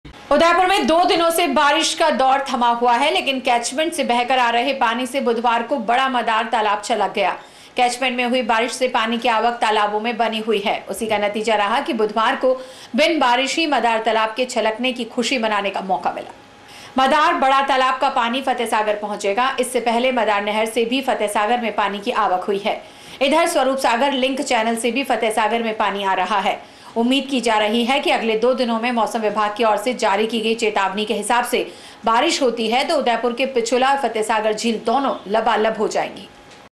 मदार छलका, मेहरबान हो मेघ तो छलक जाएगा फतहसागर।
उदयपुर में 2 दिनों से बारिश का दौर थमा हुआ है लेकिन कैचमेंट से बहकर आ रहे पानी से बुधवार को बड़ा मदार तालाब छलक गया। कैचमेंट में हुई बारिश से पानी की आवक तालाब में बनी हुई है। उसी का नतीजा रहा कि बुधवार को बिन बारिश ही मदार तालाब के छलक ने की खुशी मनाने का मौका मिला। मदार बड़ा तालाब का पानी फतेहसागर पहुंचेगा। इससे पहले मदार नहर से भी फतेह सागर में पानी की आवक बनी हुई है। इधर स्वरूप सागर लिंक चैनल से भी फतह सागर में पानी आ रहा है। उम्मीद की जा रही है कि अगले 2 दिनों में मौसम विभाग की ओर से जारी की गई। चेतावनी के हिसाब से बारिश होती है तो उदयपुर के पिछोला और फतहसागर झील दोनों लबालब हो जाएगी।